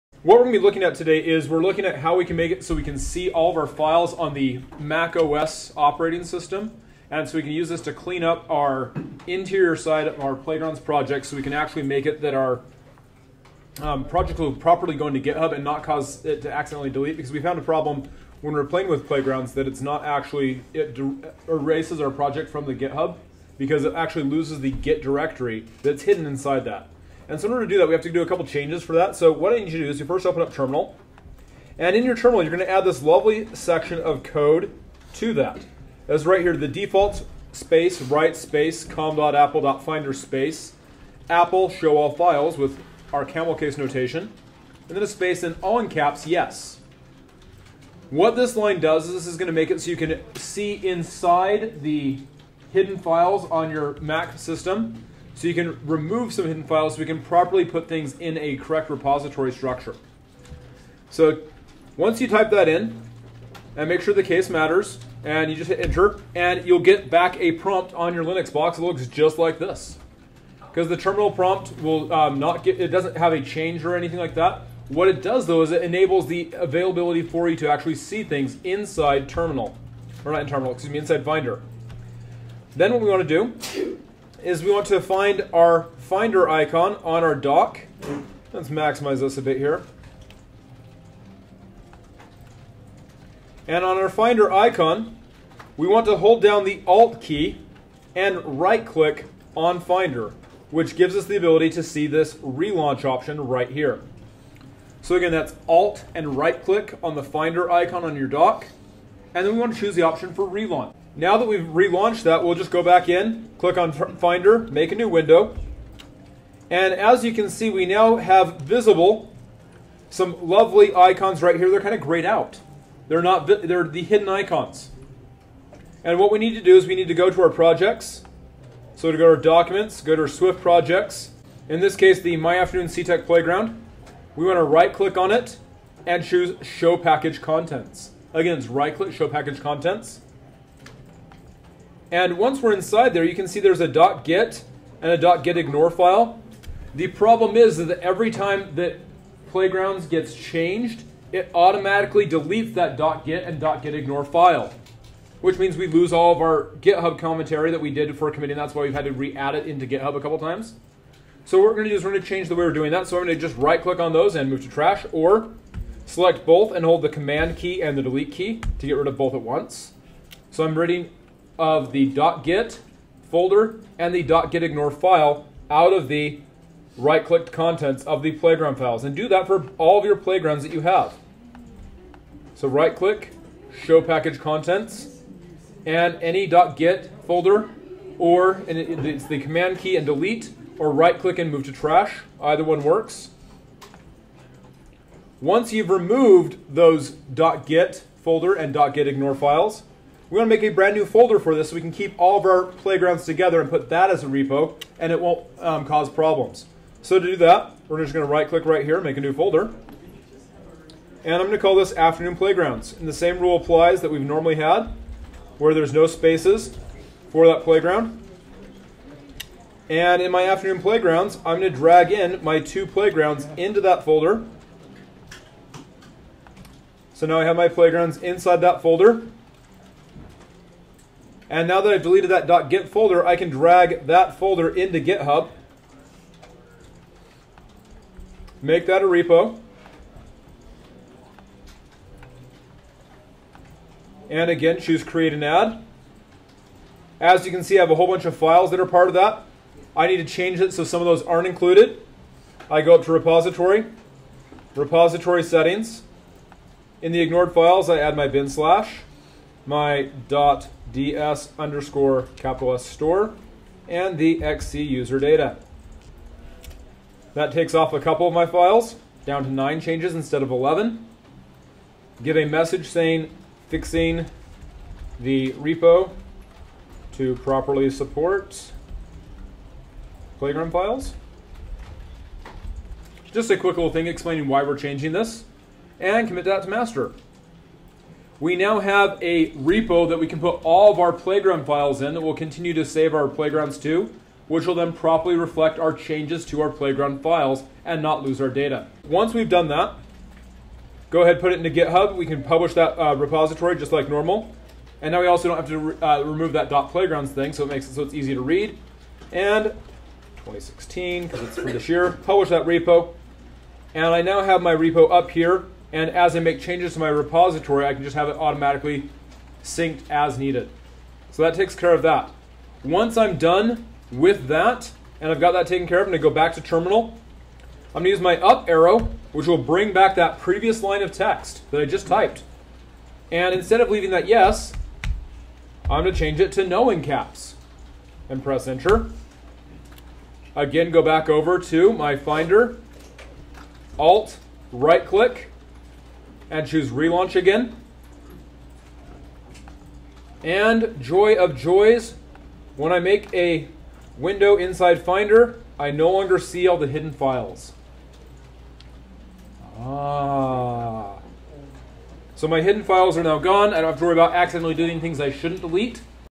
Adding Swift Playgrounds to github
Changing file visibility to correctly add Swift Playgrounds projects to a Github repository.